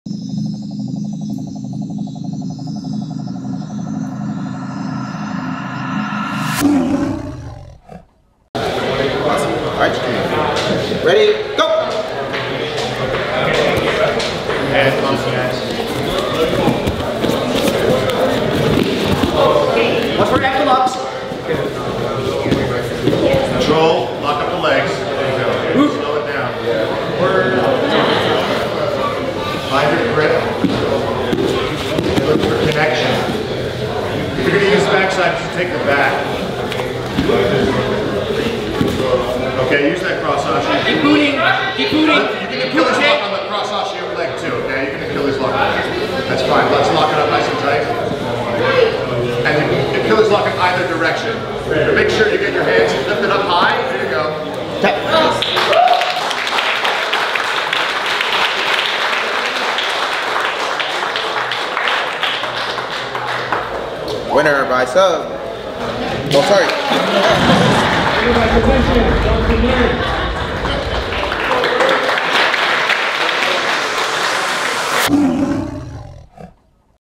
Mat Beast Open 6: NoGi Jiu Jitsu Tournament Beginner -155 Johnson Lin vs Dorian Zavala
Johnson Lin - 10th Planet HQ
Dorian Zavala - 10th Planet Thousand Oaks